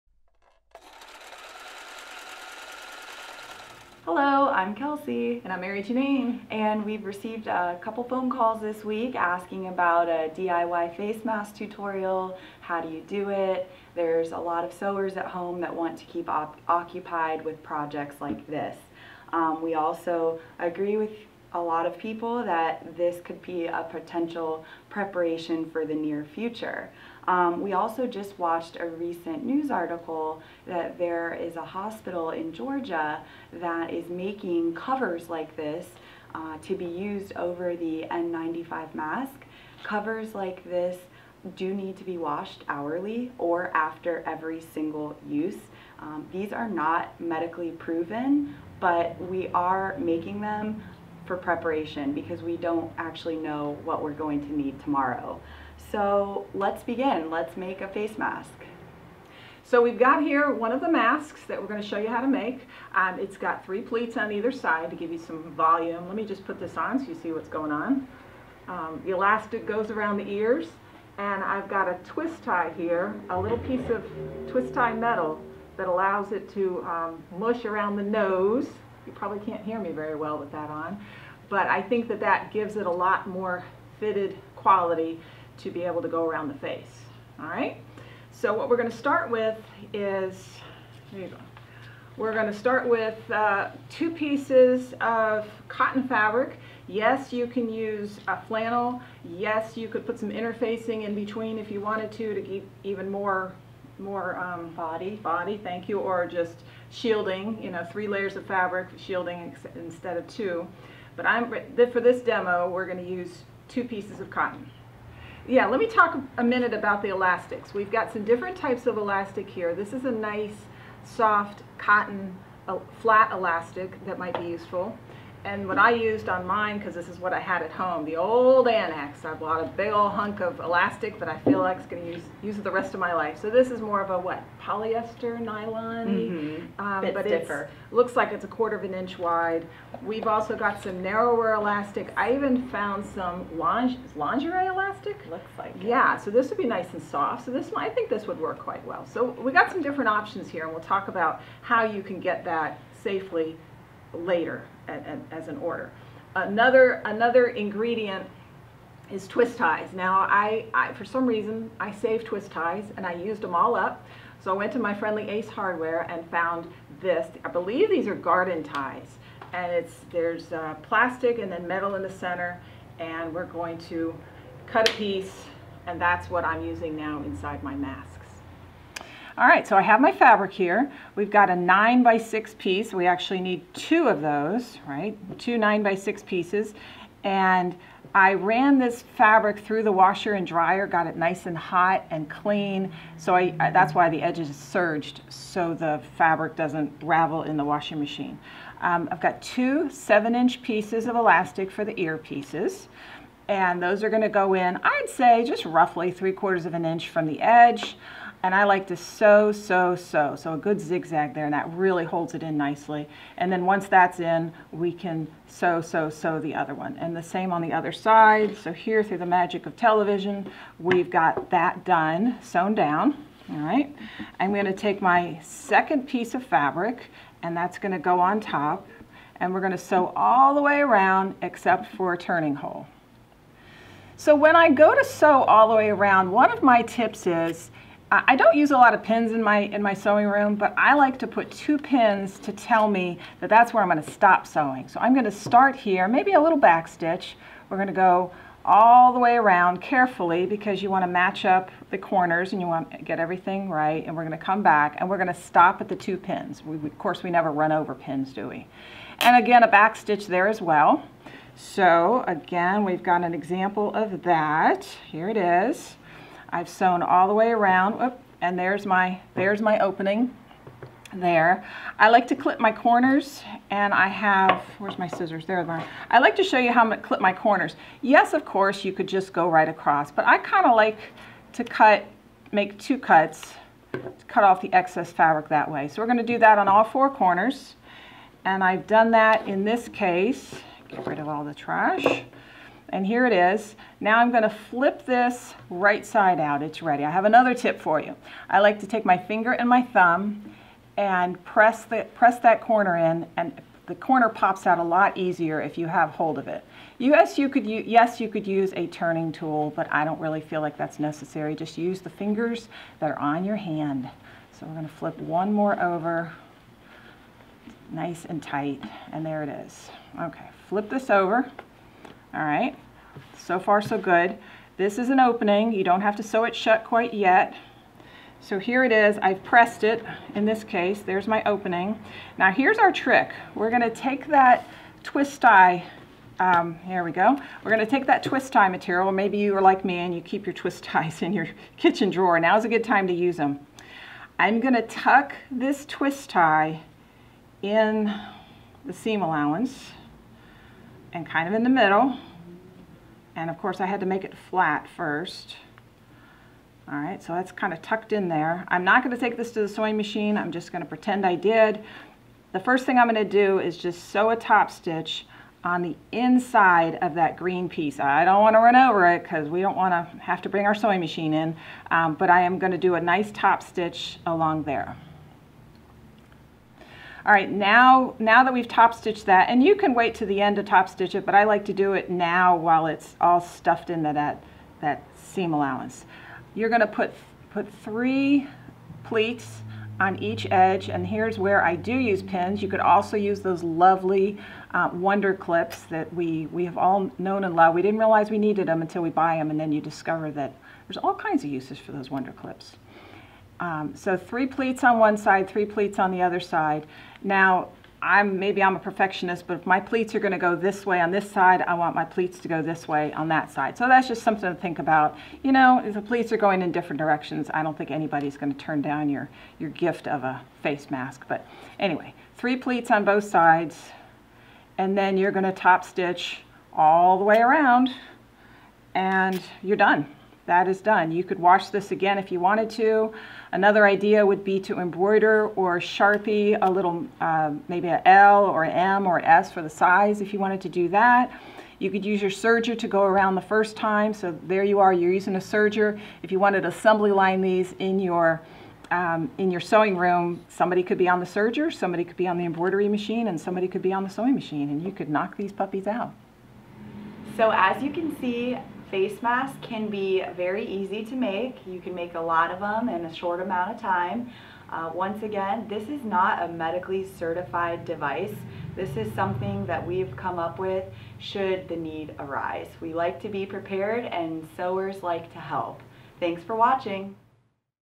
DIY Last Resort Face Mask / Face Mask Cover
We've received a number of phone calls asking about supplies for sewing face masks, so we've put together a short DIY video on the subject, knowing that our sewing community has a huge heart and wants to help.

This is a last resort face mask / face mask cover. You may have heard in the news that healthcare workers are rapidly running out of masks and are looking for ways to extend the life of their current supply.

We strongly caution our customers to follow the CDC Guidelines on masks

**We do not currently know of a local drop off for masks / mask covers but we have found two organizations out of state that are.**